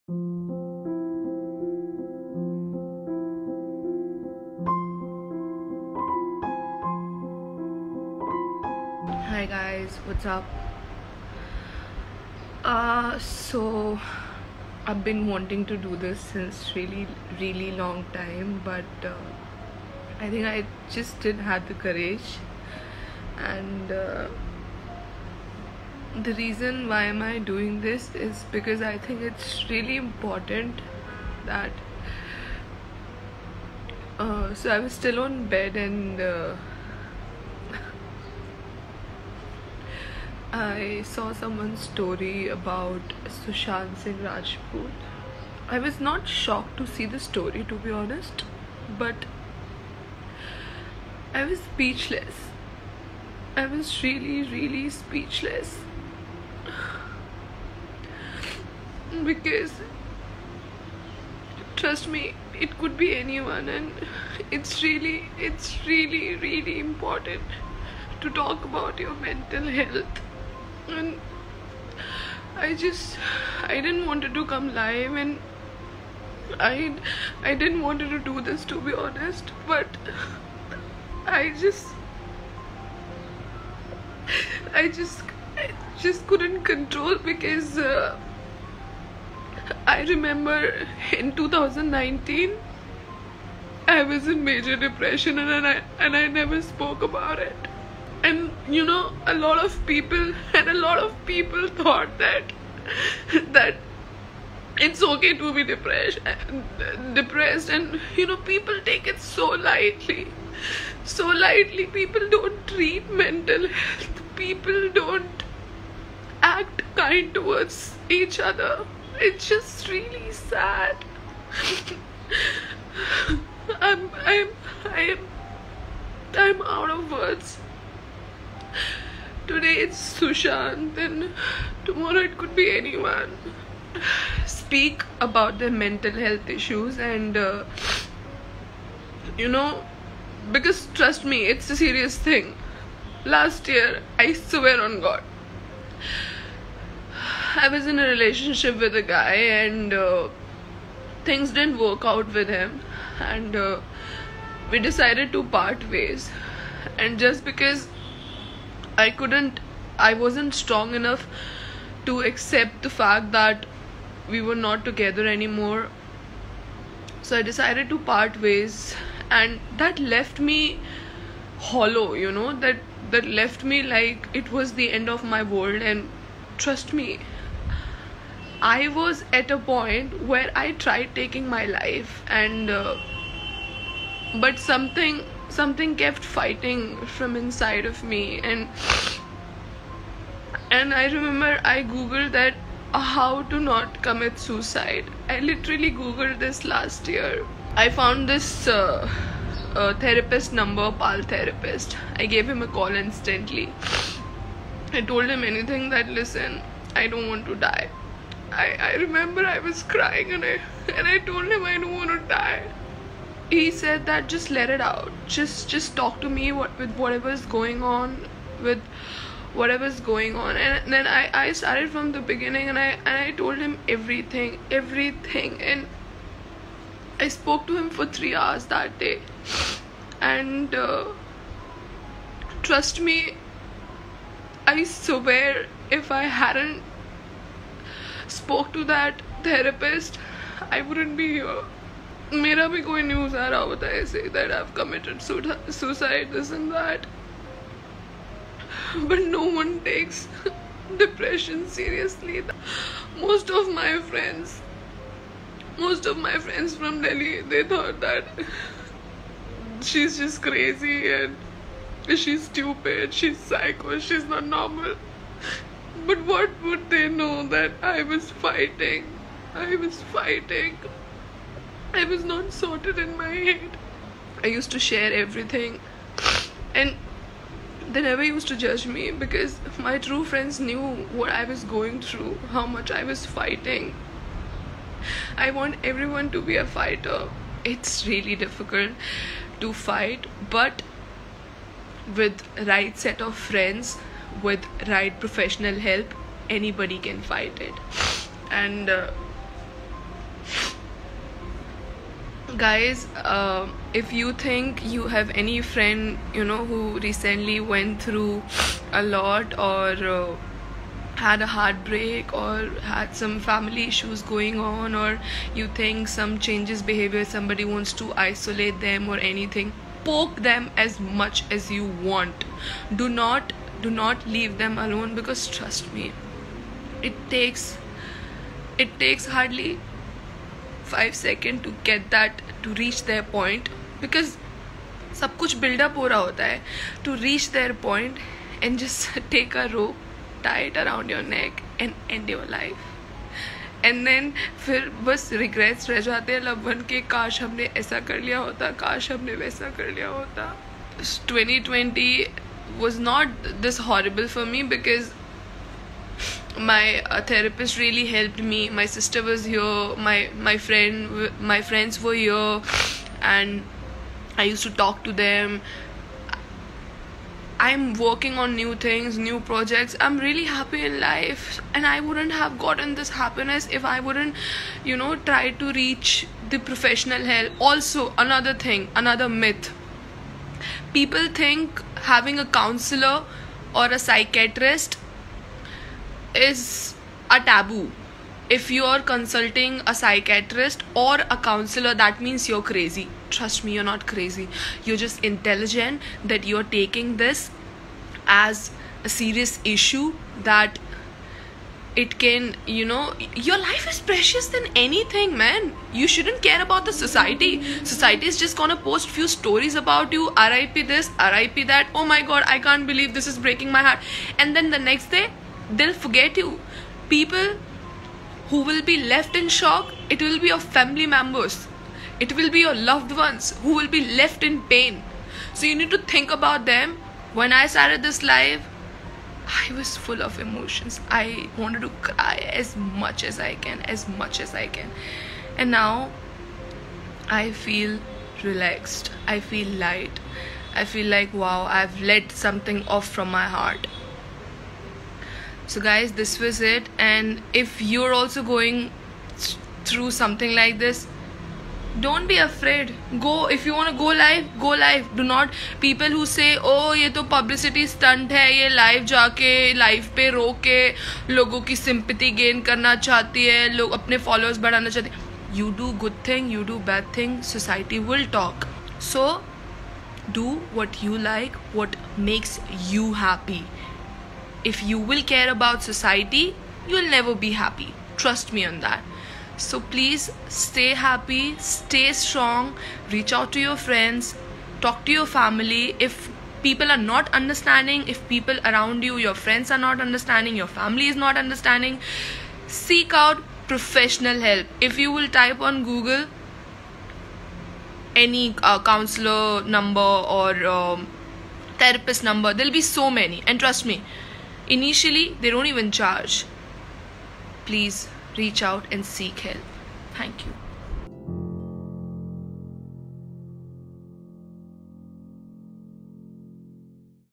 Anmol Chaudhary breaks down and opens up on depression after Sushant Singh Rajput's demise
Anmol Chaudhary has shared a video on her social media talking about her battle with depression after the demise of Sushant Singh Rajput who facing mental health issues. She shares how the news of Sushant's demise left her speechless and how important it is to talk about mental health. She opened up about her breakup with her boyfriend and how it left her with a void. She shared how she fought against depression and suicidal thoughts. She revealed how a 3-hour talk with a therapist saved her from committing suicide. She urges people to communicate with their loved ones and to seek professional help if you are facing mental health issues. Watch the video to know more.